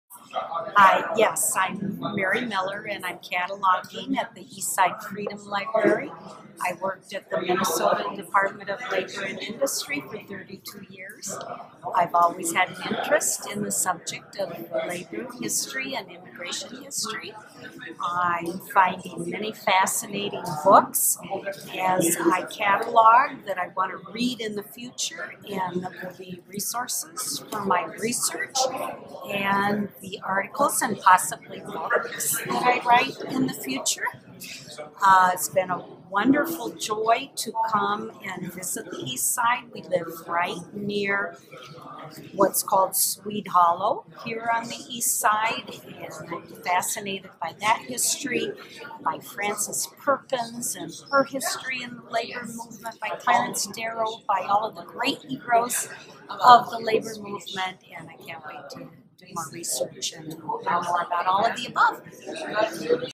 Mary Miller - Catalog Volunteer
Mary, one of our great volunteers, is helping to catalog the collection, and talks here about what she's giving her time.